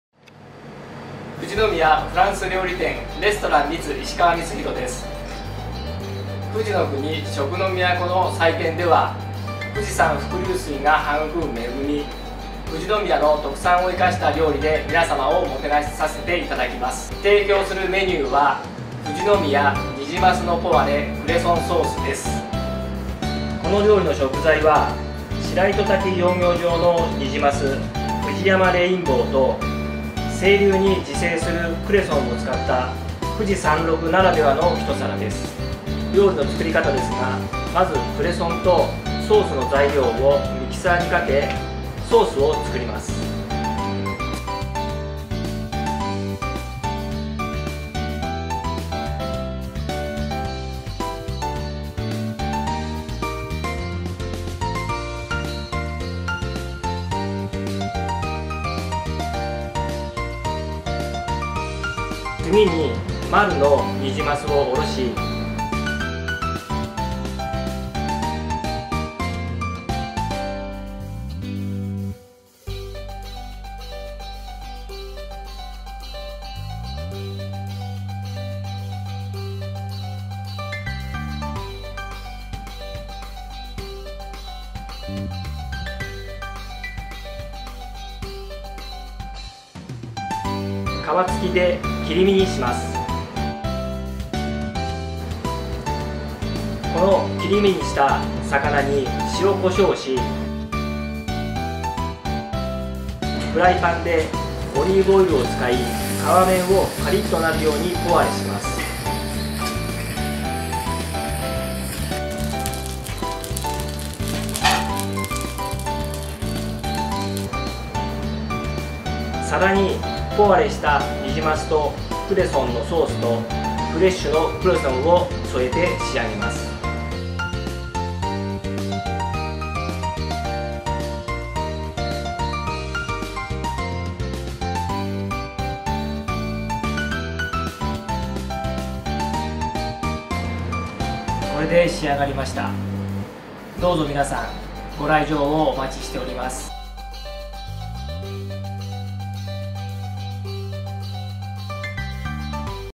ふじのくに食の都の祭典 仕事人キッチン 調理実演～富士宮虹鱒ポワレ・クレソンソース～
2016年2月20日（土）～21日（日）の2日間、静岡市内のツインメッセ静岡で開催される「ふじのくに食の都の祭典　仕事人キッチン」で販売される料理の紹介